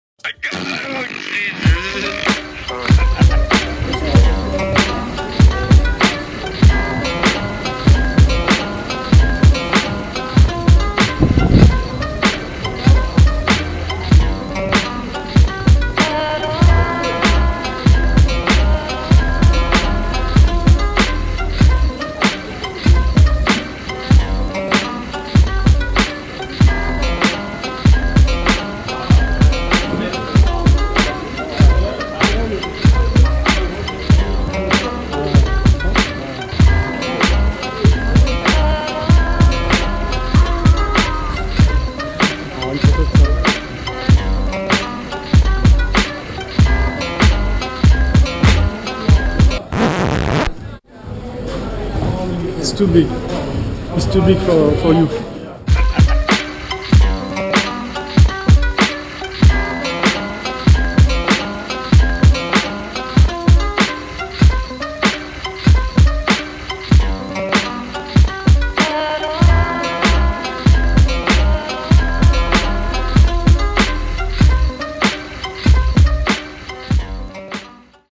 New-York Sequels series : Sneakers addict by Flight Club
Flight Club, en plein New York, un shop fait pour les sneakers du monde entier ...des baskets partout et pareil pour les casquettes... un shop de malade ... Artik et Keri ont eu mal à la tête.
Adresse : 812 Broadway, New York, between 11th and 12th

Chaine officielle de End Of The Weak France